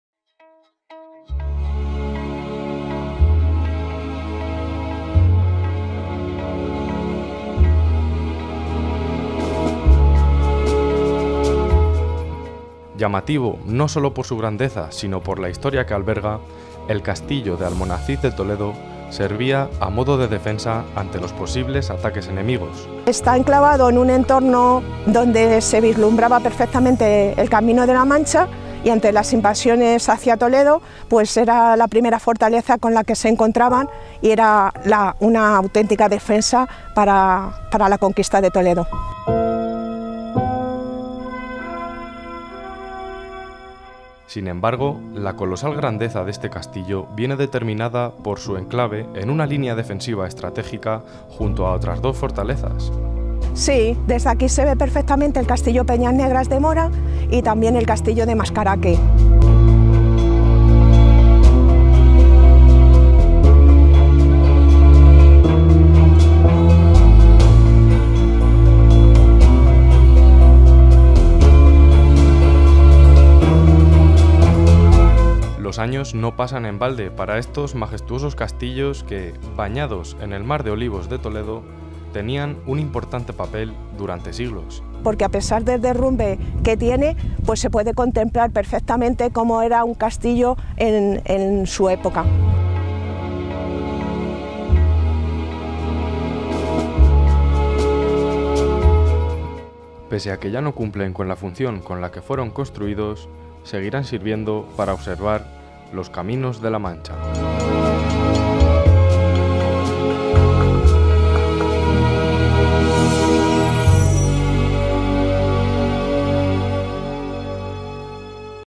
Los castillos defensivos en La Mancha toledana
De Almonacid a Peñas Negras (Mora) pasando por Mascaraque, descubrimos la importancia histórica que tuvieron estos bastiones defensivos en la provincia toledana durante la Edad Media.

Espero que disfrutéis de los Vinos de La Mancha y si os ha gustado el vídeo le deis a Like y dejéis vuestros comentarios.